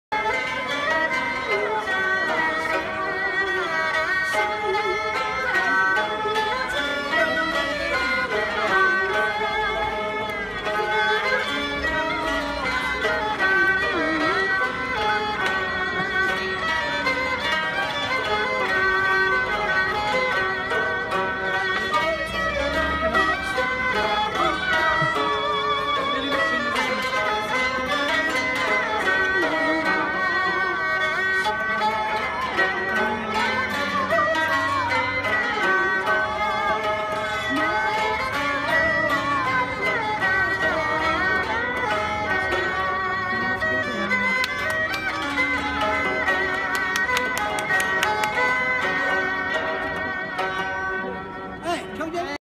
Chinese Ensemble performing in Chinatown,San Francisco,California
Chinese Ensemble playing traditional Chinese Music on the streets of Chinatown,San Francisco,California